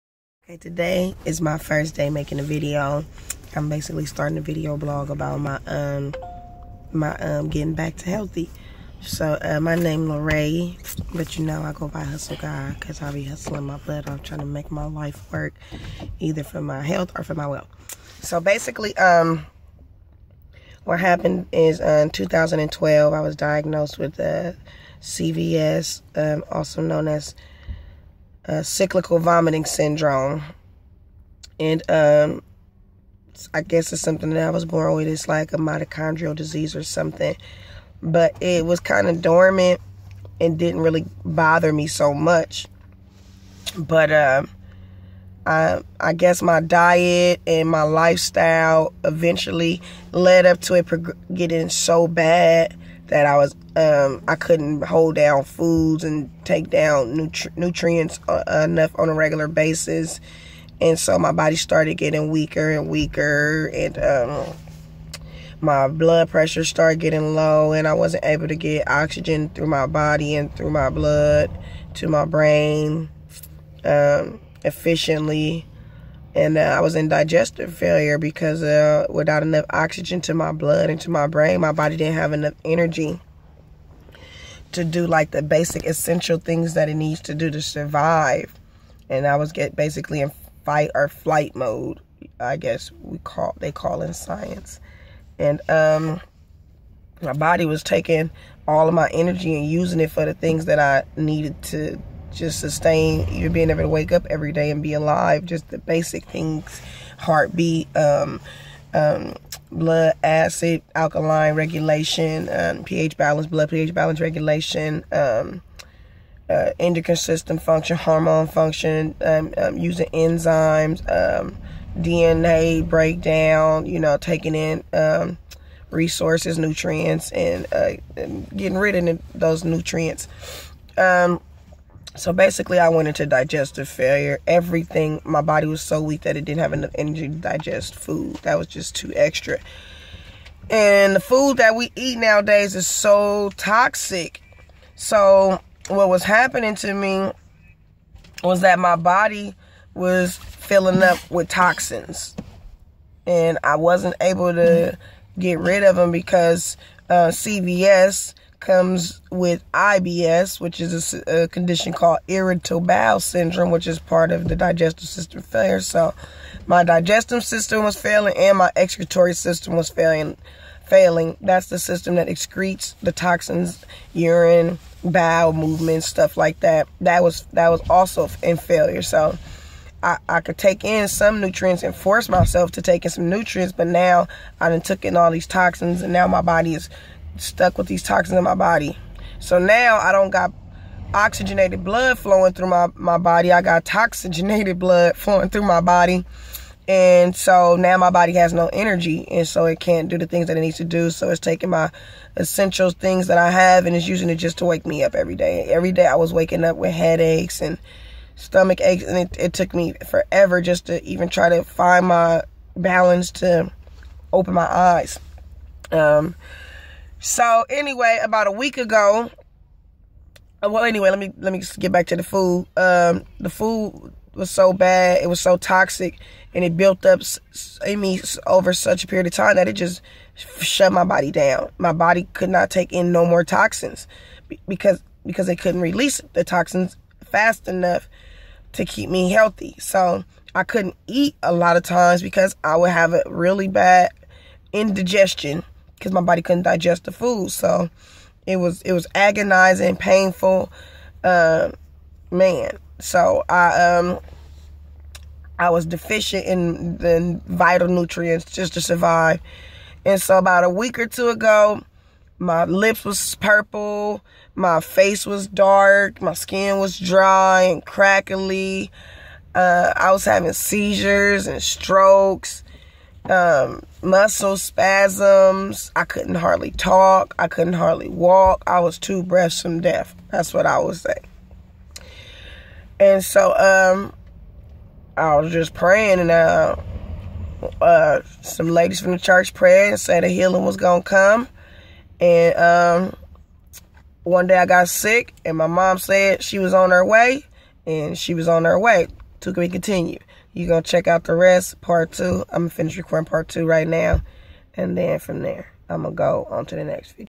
Pt.1 Dealing wit CVS/ Gerson Therapy treatment
Hustlegod describes the experience of being diagnosed with CVS and how it prigressed due to lack of nutrition in the diet and how she ultimately decided to try the Gerson Therapy